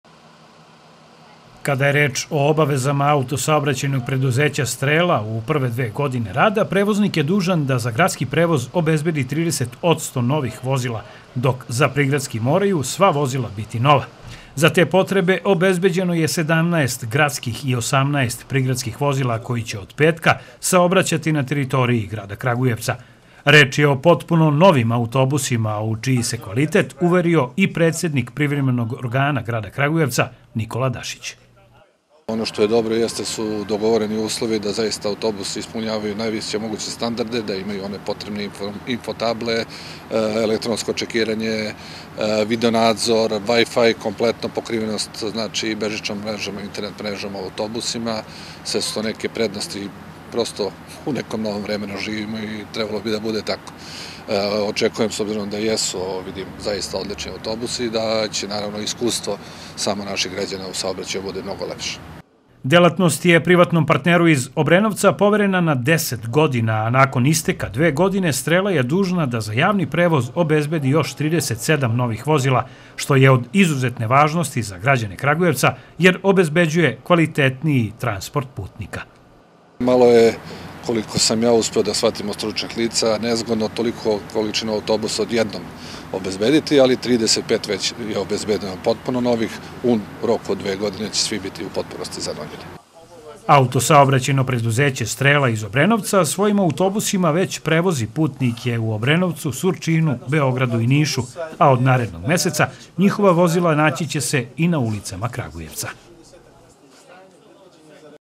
Novi autobusi od 1. decembra na kragujevačkim ulicama
Građane Kragujevca, koji koriste javni prevoz, od 1. decembra prevoziće autobusi Autosaobraćajnog preduzeća „Strela“ iz Obrenovca.